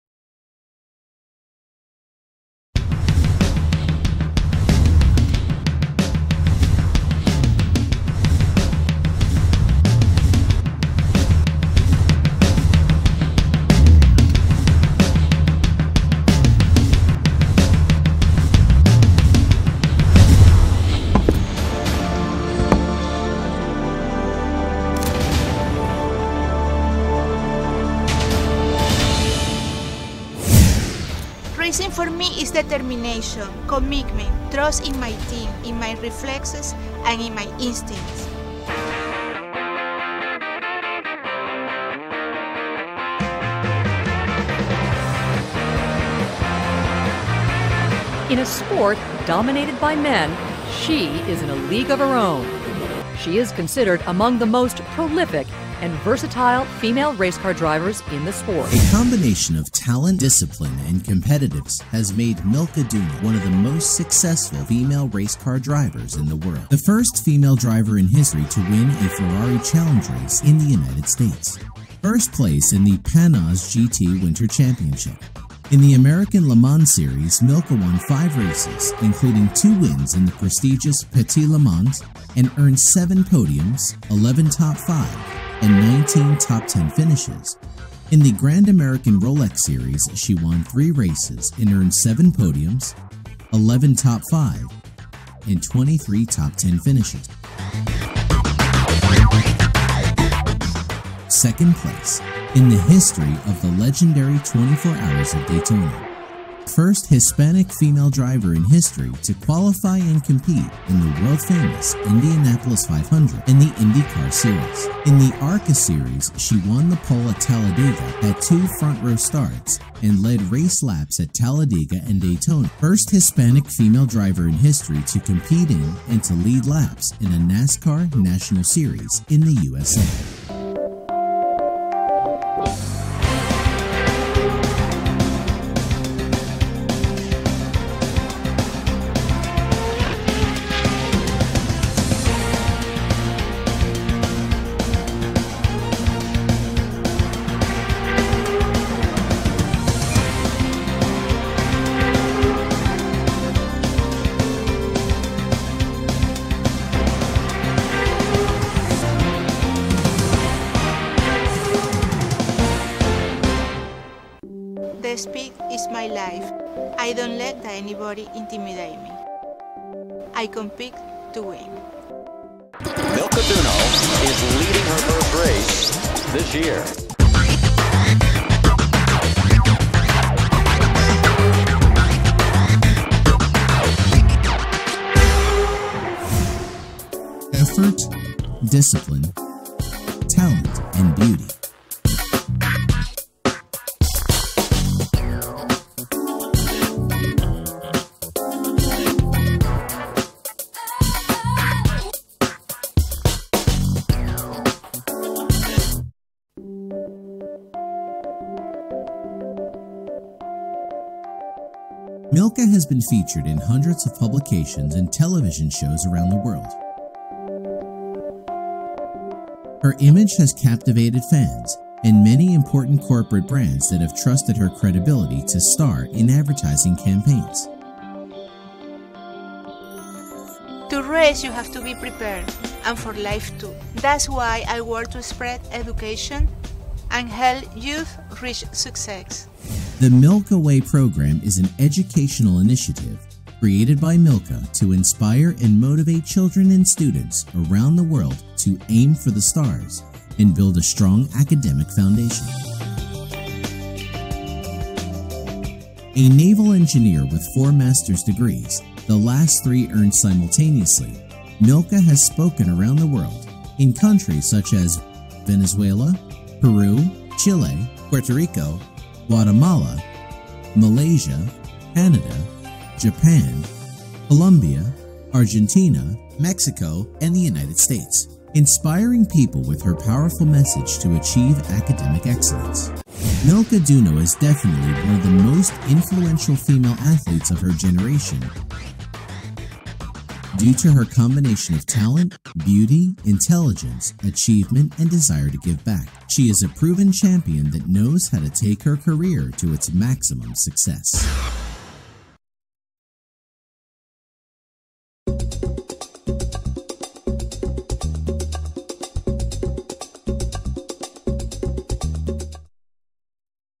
Milka Duno Official Video 2 English
Great moments in the sports career of Milka Duno